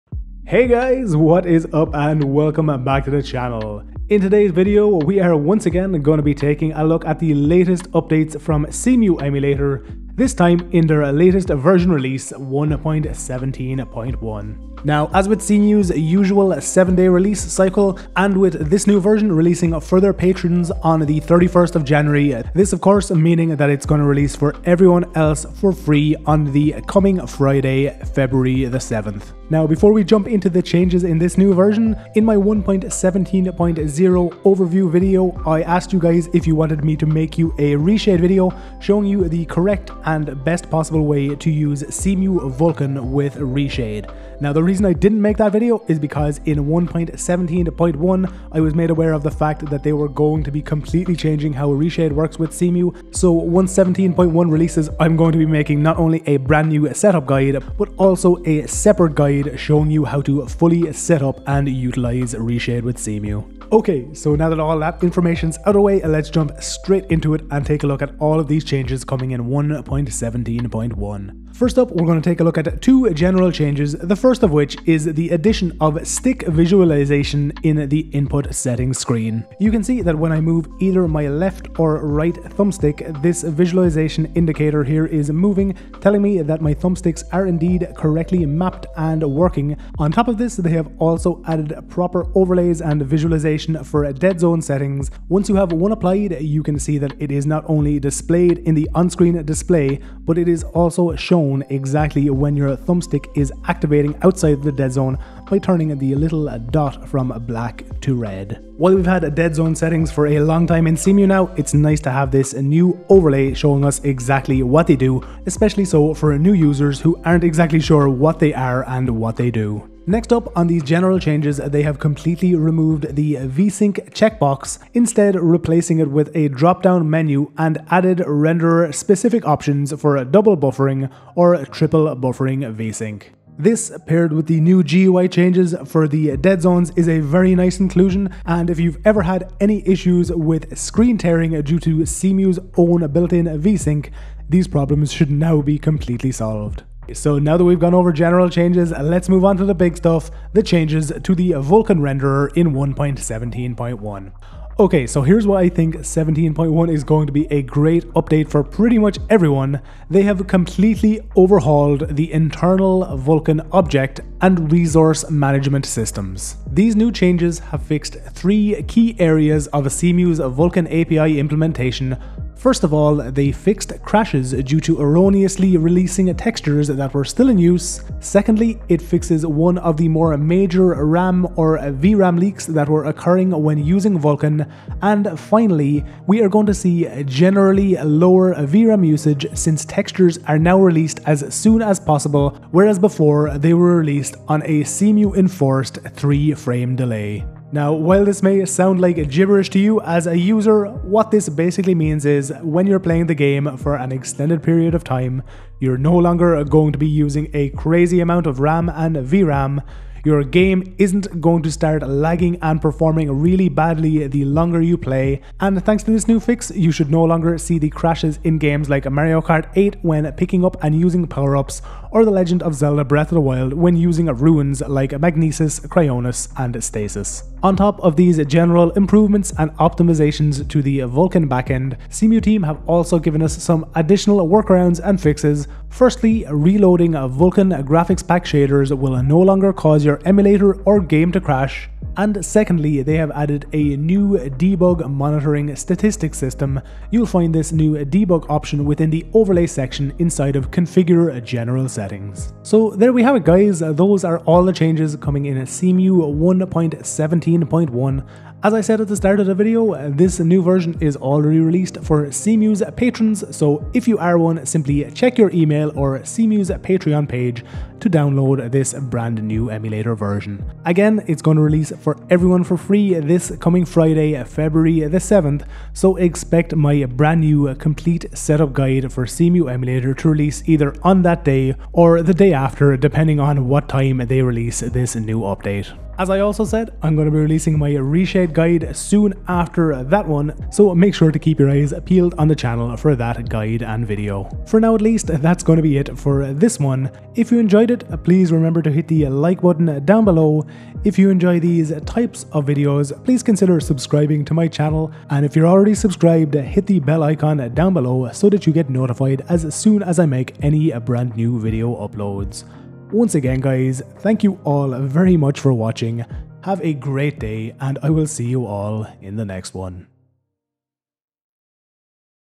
Cemu 1.17.1 Released | Vulkan Upgrades, New Controller GUI, Reshade Changes & More
Cemu 1.17.1 is now released,. This new Cemu Version brings with it a host of New Updates to the Vulkan Backend, General Emulator GUI Changes and improvements to Stability

Cemu 1.17.1 Public Release will take place on Friday February 7th

► My PC Specs:
16gb DDR4 3200Mhz RAM
GTX 1080ti 11gb
256gb NVME M.2 SSD